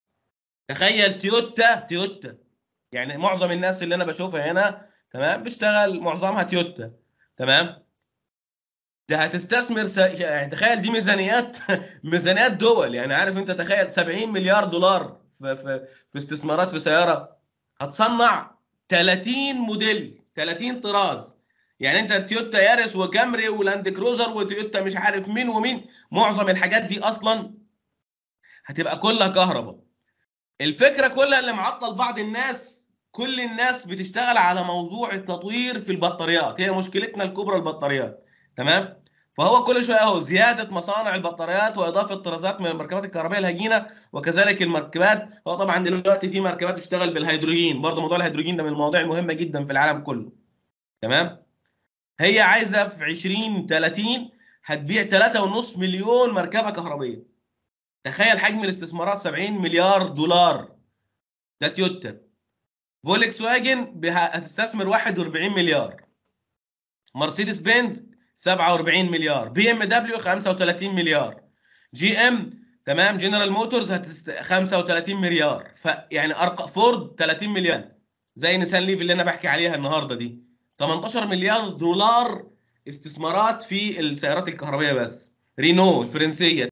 "الاستثمار في السيارات الكهربائية: نمو هائل وتوجهات المستقبل"
"استكشف كيف يتزايد حجم الاستثمارات في قطاع السيارات الكهربائية وكيف تؤثر هذه الاستثمارات على المستقبل التكنولوجي والصناعي. في هذا الفيديو، سنلقي نظرة على البيانات الرئيسية والاتجاهات المستقبلية التي تدفع النمو في هذه الصناعة الرائدة.